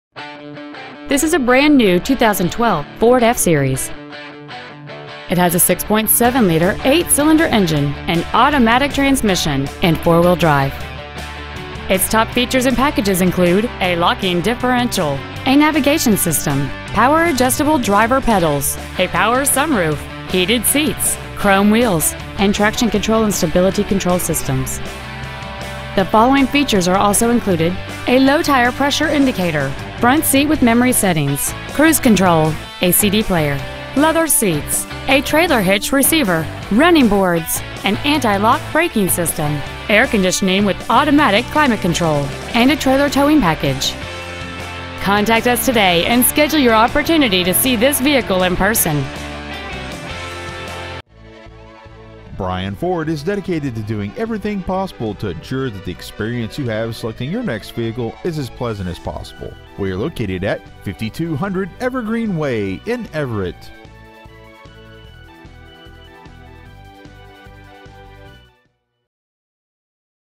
2012 FORD F-350 Everett WA
Year : 2012
 Make : FORD
 Model : F-350
 Engine : 6.7L V8 32V DDI OHV TURBO DIESEL
 Trans . : 6-SPEED AUTOMATIC
 Exterior : TUXEDO BLACK METALLIC

 Interior : CHARCOAL
 Stock : 129145